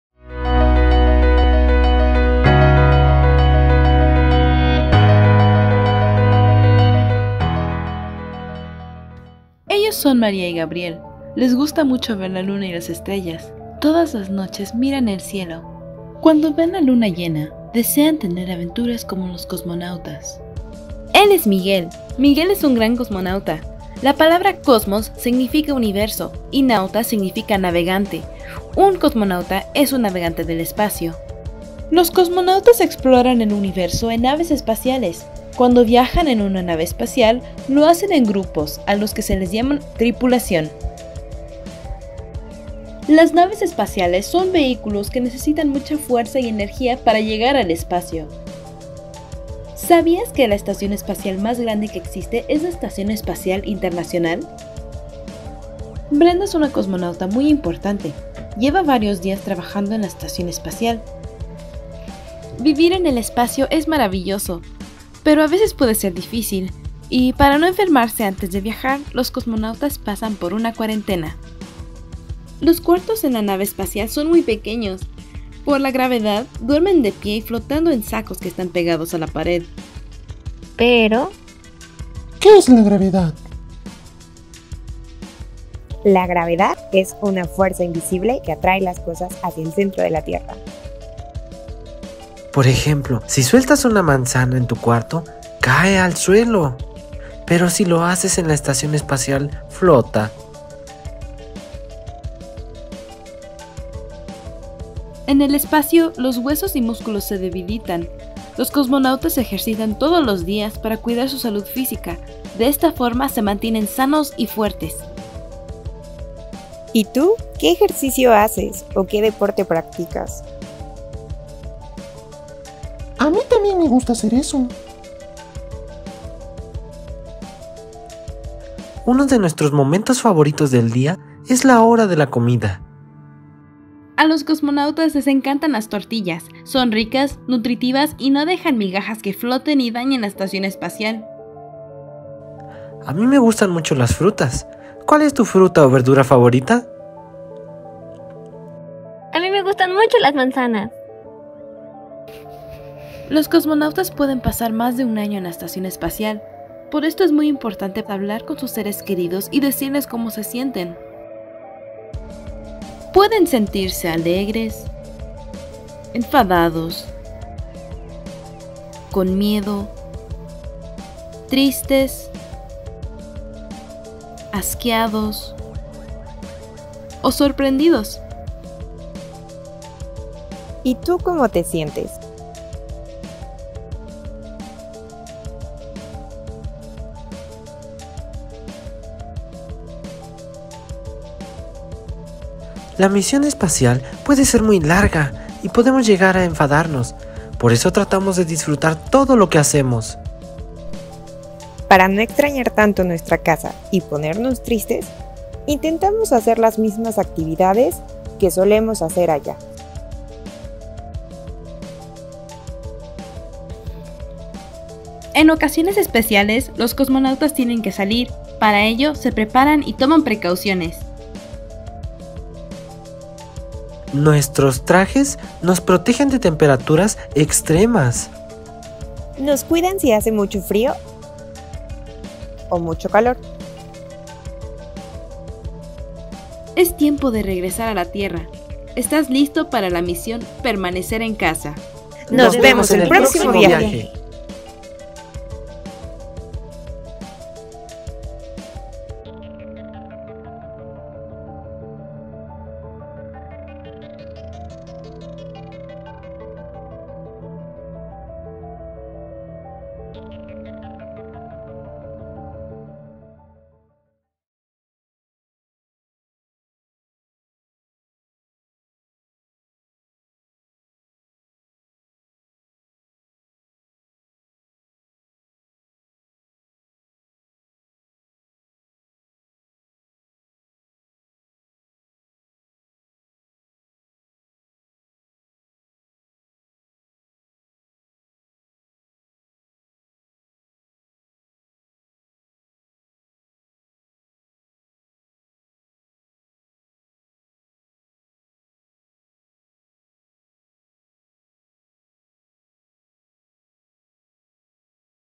De viaje en el espacio. Herramientas sencillas para vivir el confinamiento
Brenda y Miguel nos dan un recorrido por las diferentes actividades que realizan las y los cosmonautas durante su estancia en el espacio.
Adaptación del libro infantil "De viaje en el espacio". Herramientas sencillas para vivir el confinamiento", aborda desafíos psicosociales que experimentaron niñas y niños durante el confinamiento por la pandemia de Covid-19 así como estrategias prácticas para sobrellevarlos.